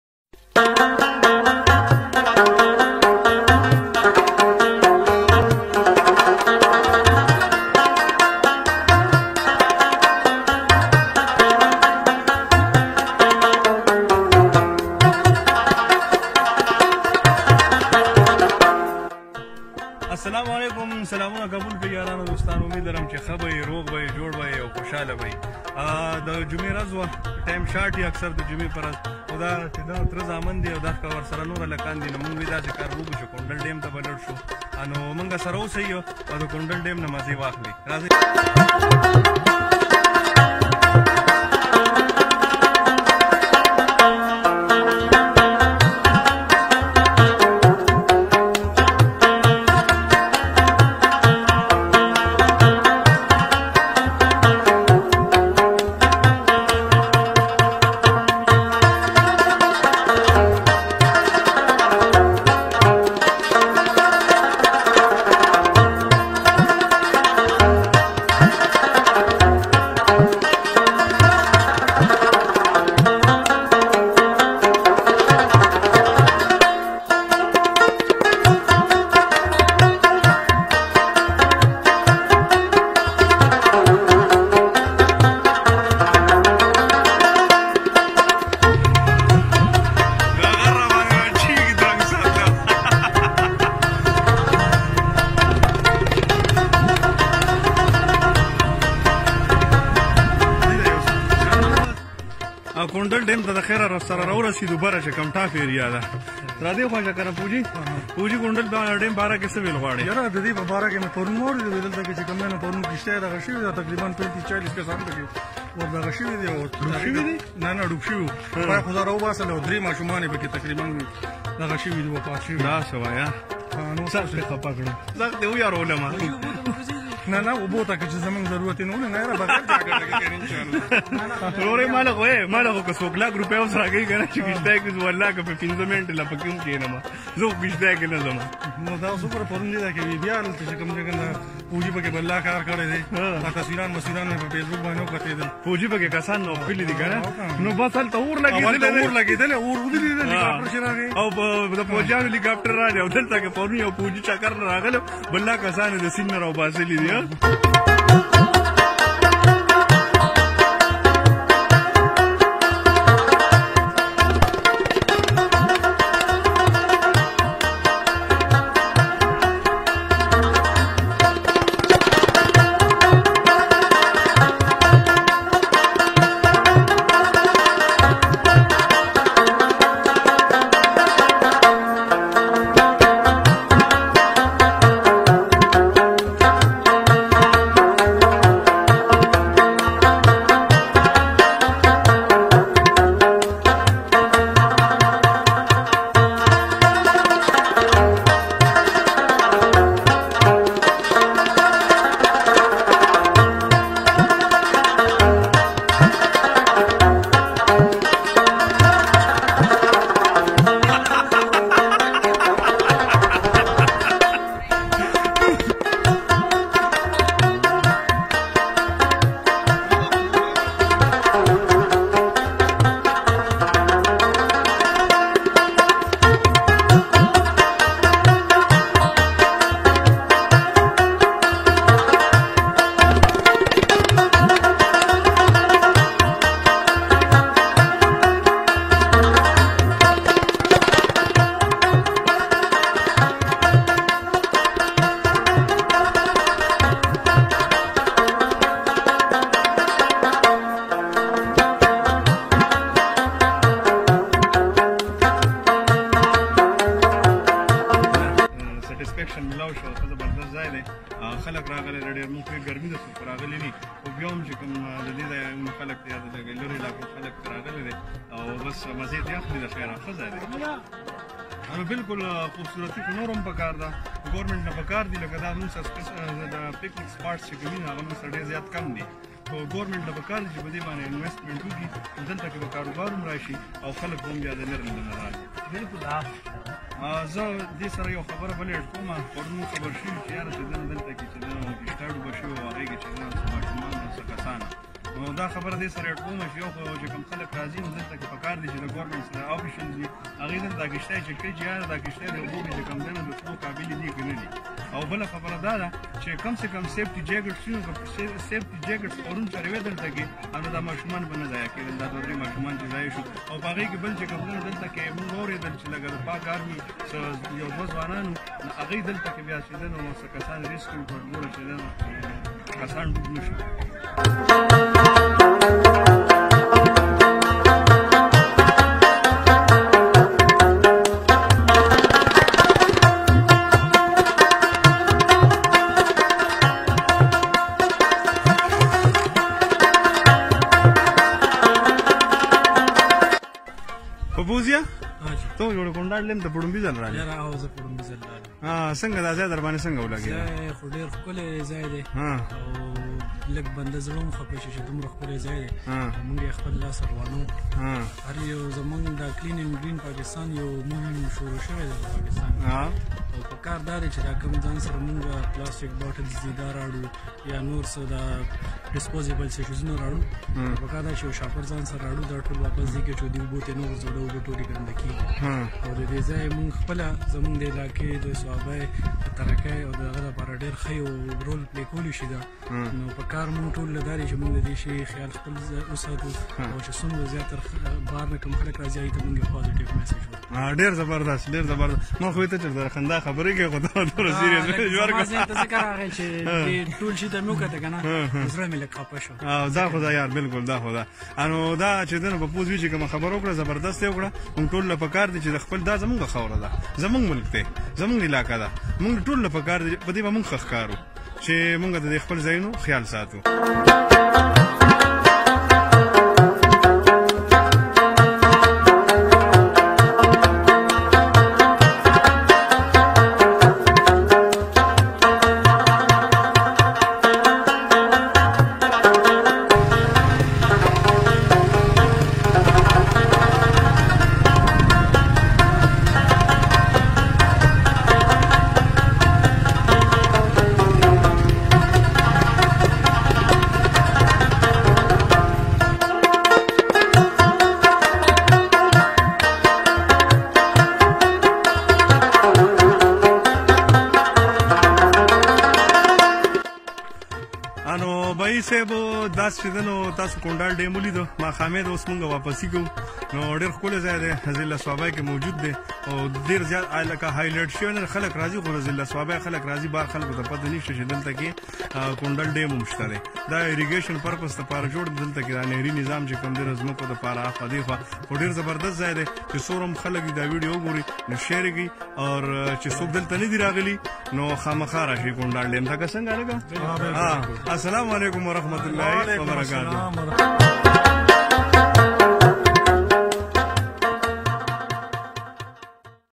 KUNDAL DAM | SWABI | KPK |PAKISTAN
Small dam located in swabi district near Janda village.
purpose of the video to attract tourists